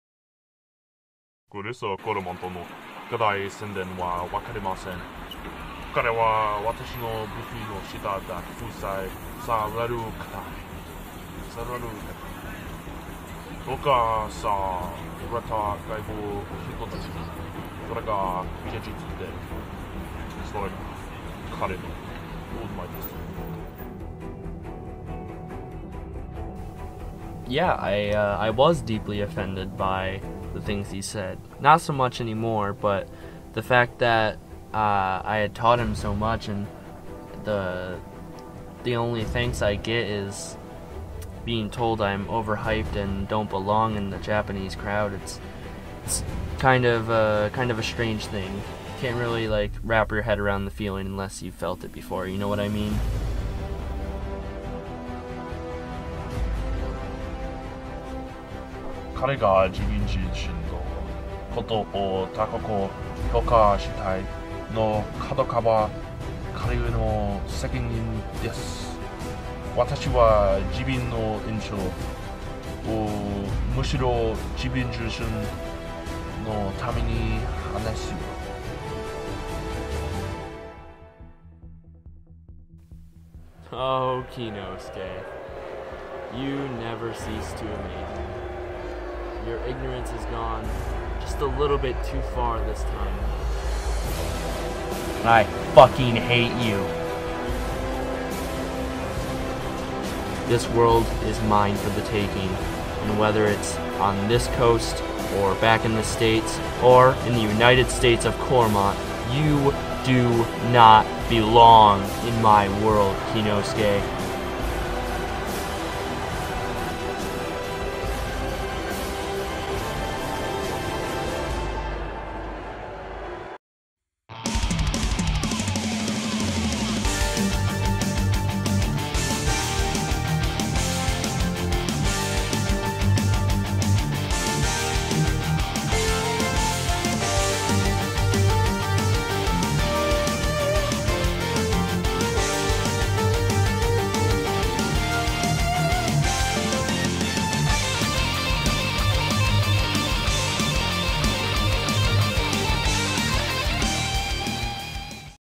Chris Corremont vs KINNOSUKE [PV]
In the last match of Chris Corremont's Asian Adventure Tour, he runs into the Rapid Wilderbeast and rival of FENRIR Shingo. The two were friends, but in a press junction KINNOSUKE told reporters that he did not believe the hype around Chris Corremont. The two are set to face off in GENESIS' biggest event of the year! ZA SUNDEDOME! (The ThunderDome)